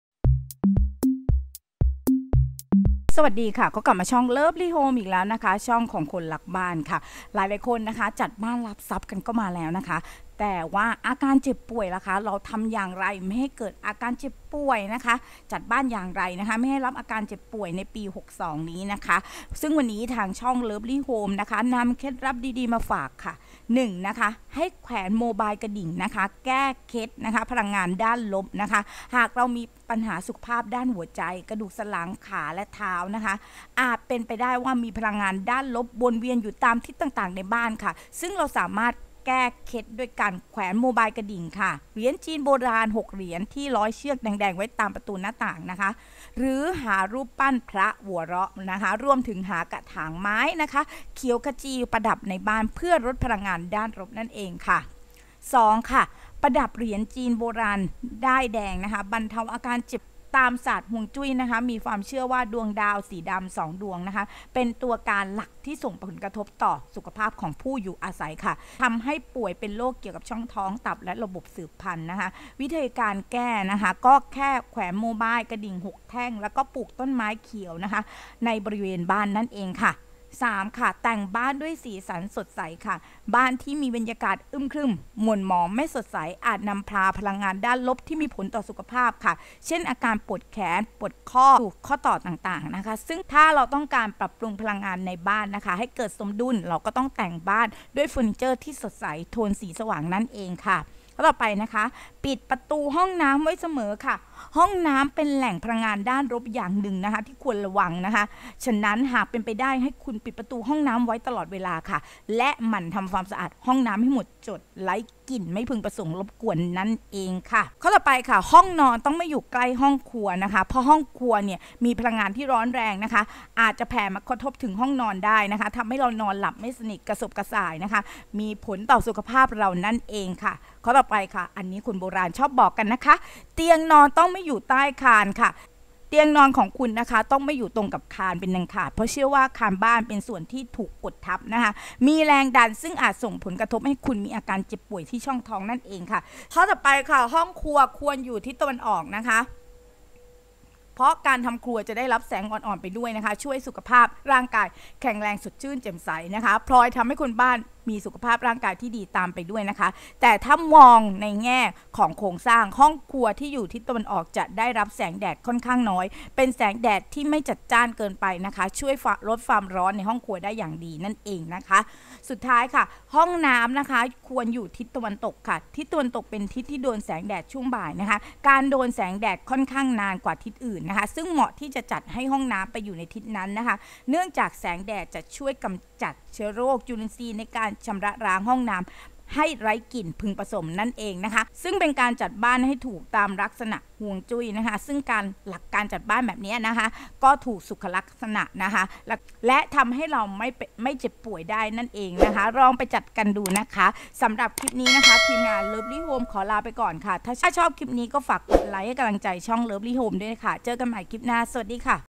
แชร์จัดบ้านตามหลักฮวงจุ้ยให้สุขภาพไม่เจ็บไม่ไข้ ตามหลักฮวงจุ้ยปี62 l lovely home
สวัสดีค่าวันนี้ทีมงานlovely homeนำเคล็ดลับดีๆๆในการจัดบ้านตกแต่งบ้านมาฝากอีกแล้วค่า ว่าจัดบ้านอย่างไรให้สุขภาพดีไม่เจ็บไม่ไข้ในปี2562นี้เราไปดูกันค่า